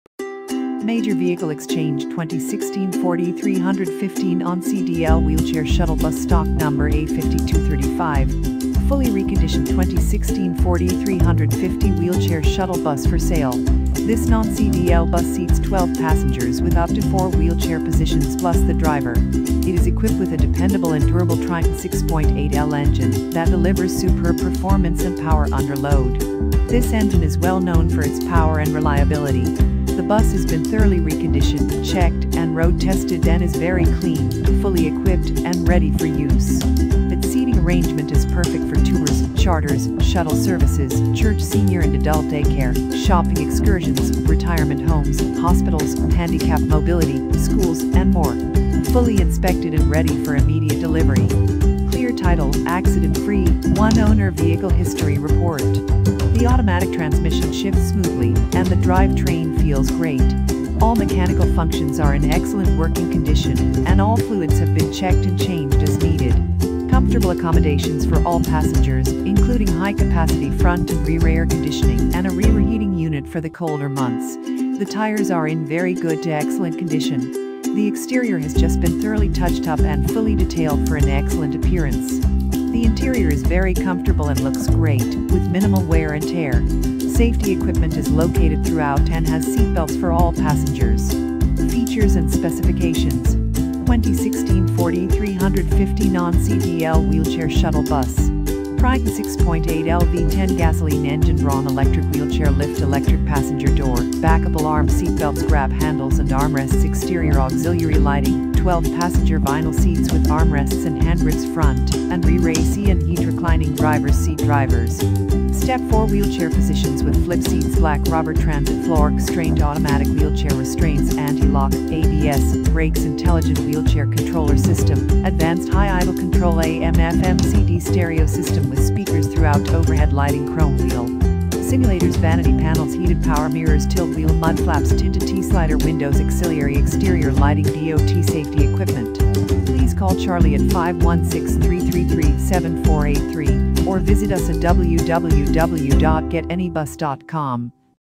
2016 Ford E350 Non CDL Wheelchair Shuttle Bus For Sale Fully Reconditioned !
Fully reconditioned 2016 Ford E350 wheelchair shuttle bus for sale. This Non-CDL bus seats 12 passengers with up to 4 wheelchair positions plus the driver. It is equipped with a dependable and durable Triton 6.8L engine that delivers superb performance and power under load. This engine is well known for its power and reliability. The bus has been thoroughly reconditioned, checked, and road-tested and is very clean, fully equipped, and ready for use. Its seating arrangement is perfect for Tours, Charters, Shuttle Services, Church, Senior and Adult Daycare, Shopping Excursions, Retirement Homes, Hospitals, Handicapped Mobility, Schools, and more. Fully inspected and ready for immediate delivery. Clear title, accident-free, one-owner vehicle history report.

The automatic transmission shifts smoothly and the drive train feels great. All mechanical functions are in excellent working condition, and all fluids have been checked and changed as needed. Comfortable accommodations for all passengers, including high-capacity front and rear air conditioning and a rear heating unit for the colder months. The tires are in very good to excellent condition. The exterior has just been thoroughly touched up & fully detailed for an excellent appearance. The interior is very comfortable and looks great, with minimal wear and tear. Safety equipment is located throughout and has seatbelts for all passengers.

FEATURES & SPECIFICATIONS:

2016 Ford E-350 Non-CDL Wheelchair Shuttle Bus

Please call Charlie at or visit us for our full inventory of reconditioned shuttle buses and wheelchair buses/vans for sale!